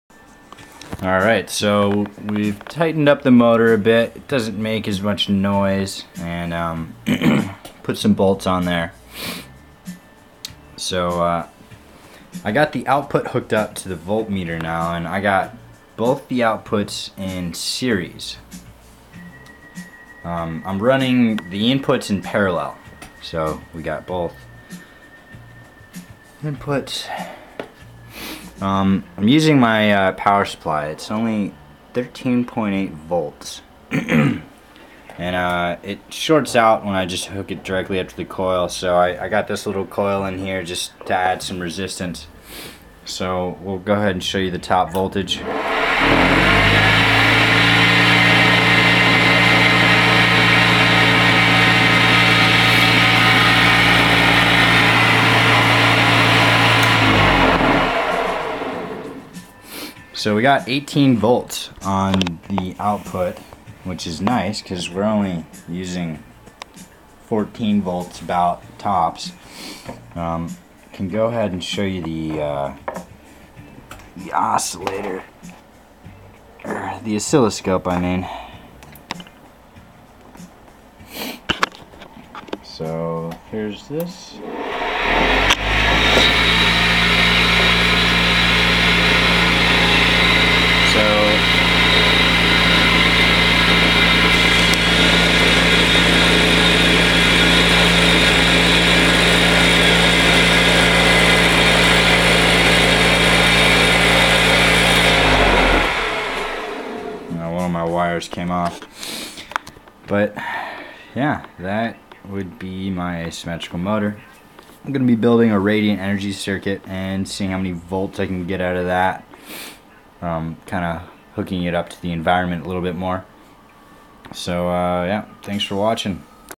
Asymmetric Scooter motor with output voltage
running on 13.8 volts parallel input with 18.0 volts series output